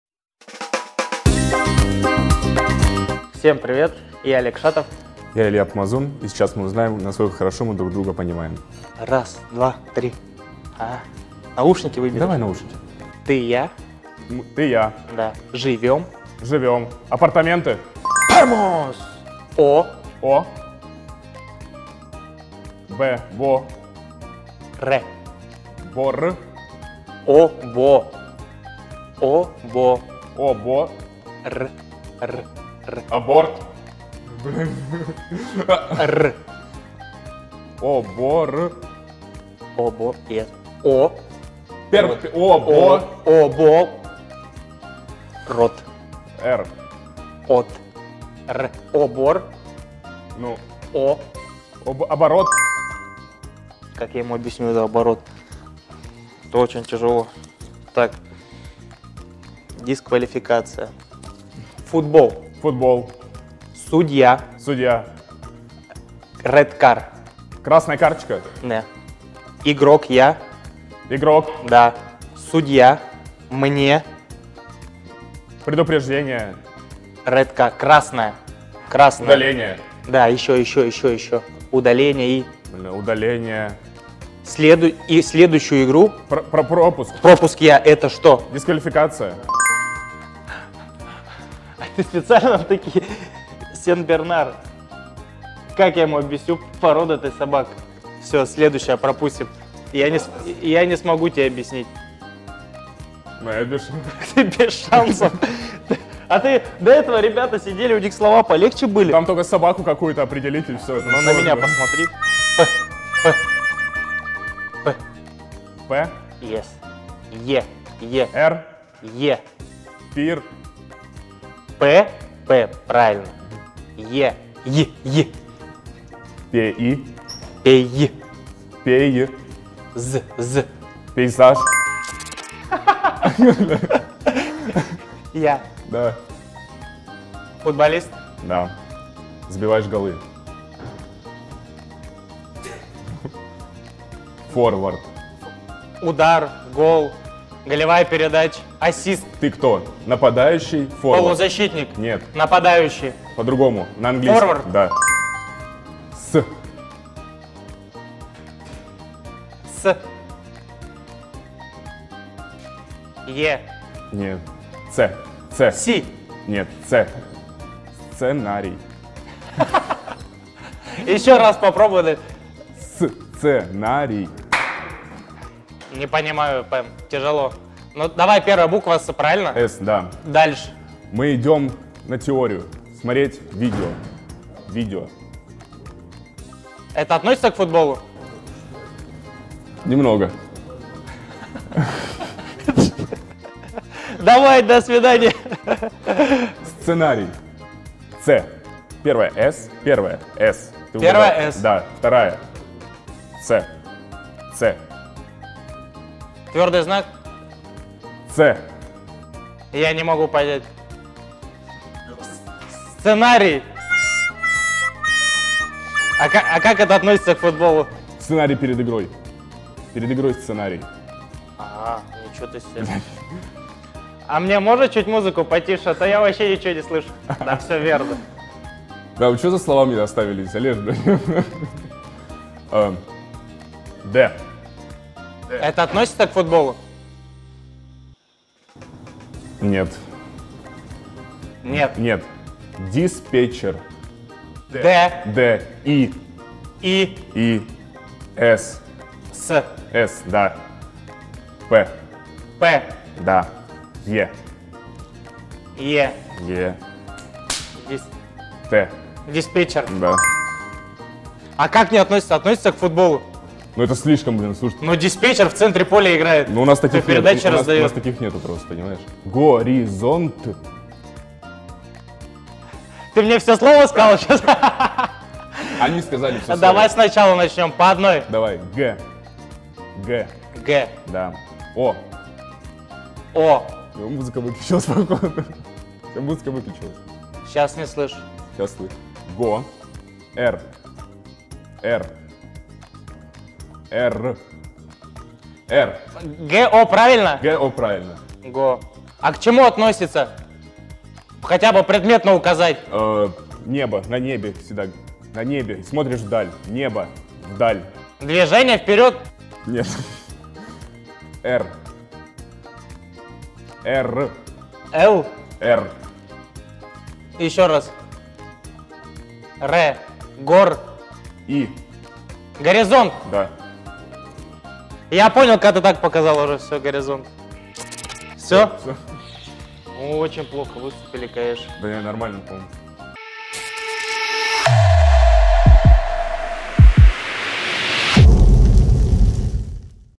ПОЙМИ МЕНЯ // Шатов VS Помазун
Футболисты «Урала» до отпуска отлично поработали не только на поле. Чтобы болельщики не заскучали в декабре, наши парни подготовили разнообразные весёлые видео. Публикуем первое из них. Илья Помазун и Олег Шатов по очереди затыкают себе уши, чтобы проверить, как хорошо они понимают друг друга.